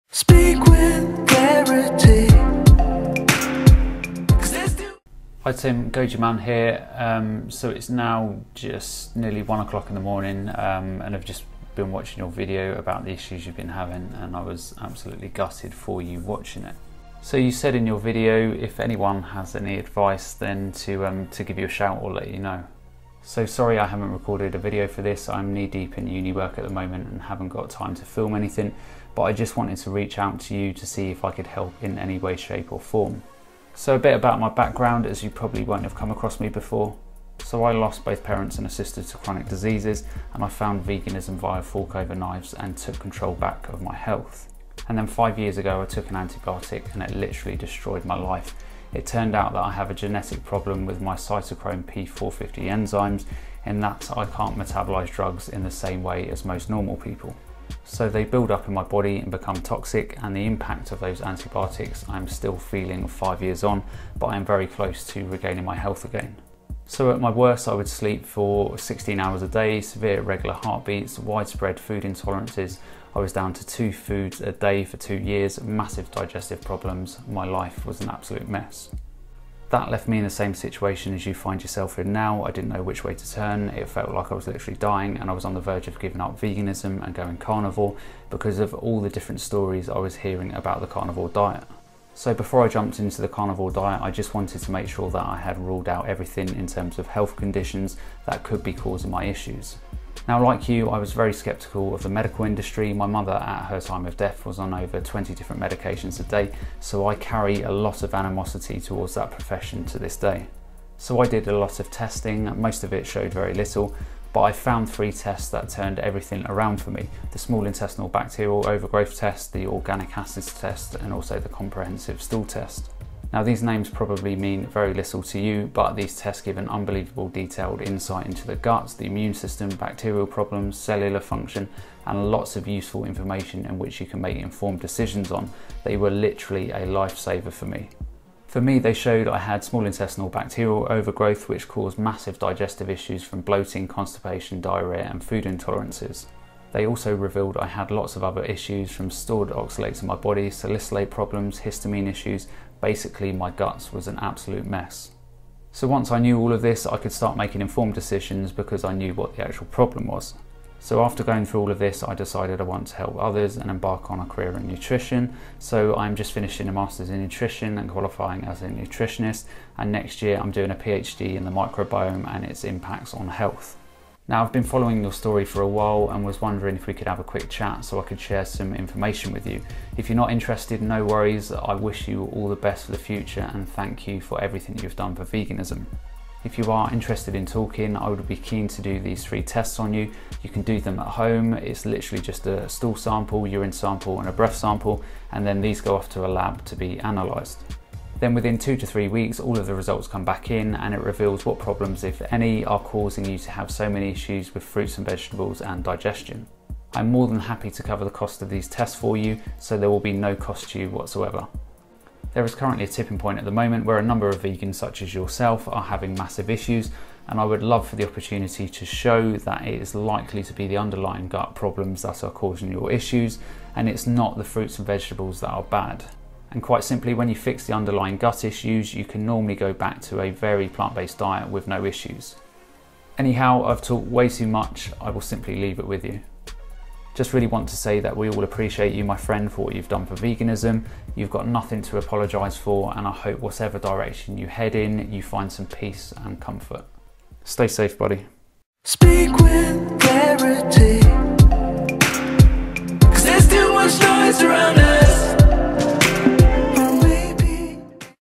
Dear Tim Shieff......
In this video I reach out to Tim Sheiff.

As always if you have a question for me then askgojiman in the comments below or send your video question through to